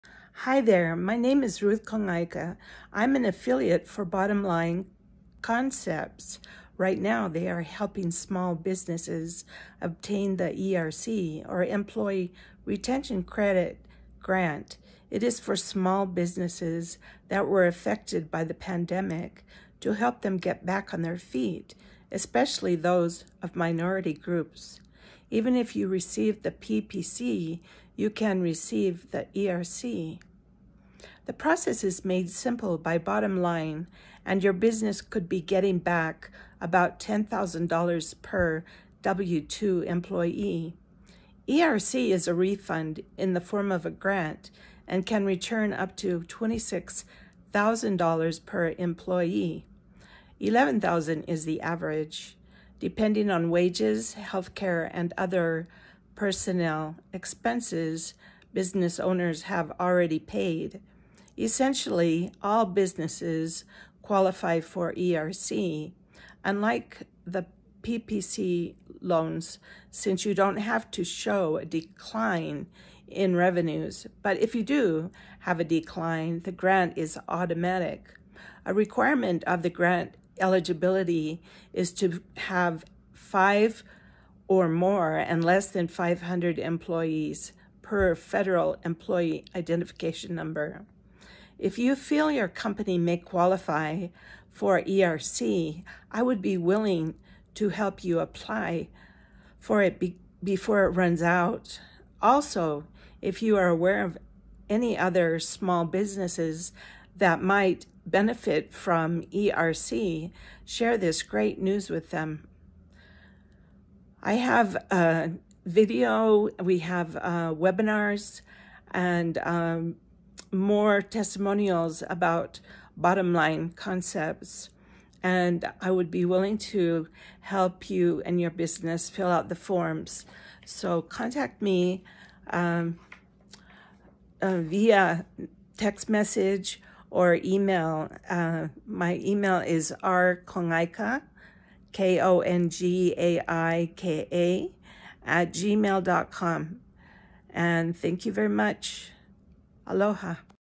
Female represents BottomLine Concepts gives grants to qualified Small Businesses available 2022
Female represents BottomLine Concepts who gives grants to qualified Small Businesses available 2022

Right now they are helping small businesses obtain the ERC (Employee Retention Credit) grant. It is for small businesses that were affected by the Pandemic to help them get back on their feet, especially those of minority groups.
The process is made simple by bottom line, and your business could be getting back about $10,000 per W-2 employee. ERC is a refund in the form of a grant and can return up to $26,000/employee ($11,000 is the average) depending on wages, health care, and other personnel expenses business owners have already paid.

The link below has much more information including a video, FAQs, and testimonials for BottomLine. Here is the form if you want to see if you qualify. Don't miss out on this timely grant to help your small business grow!